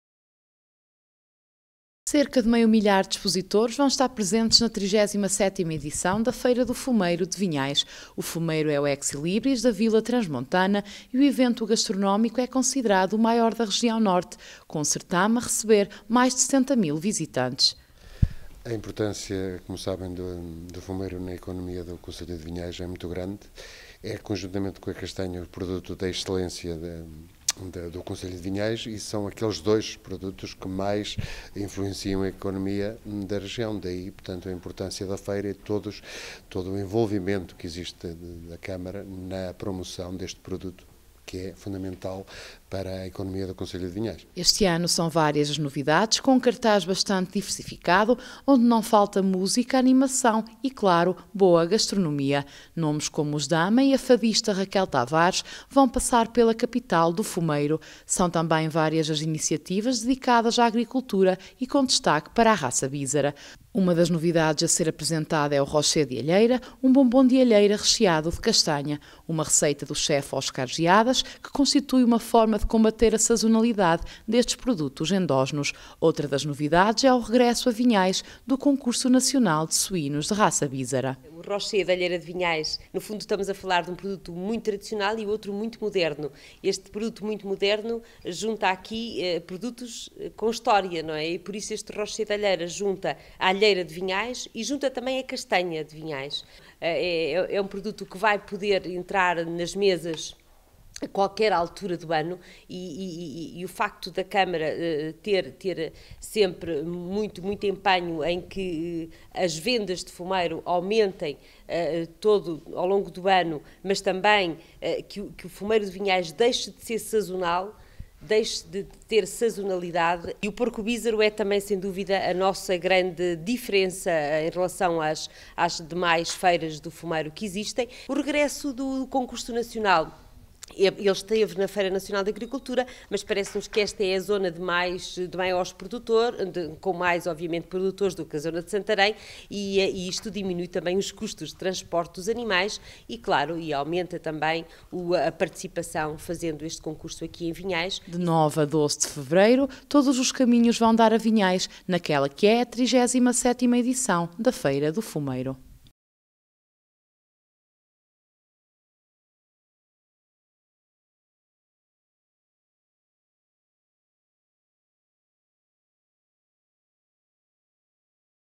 Feira do Fumeiro de Vinhais
Capital do Fumeiro recebe mais uma edição do tradicional certame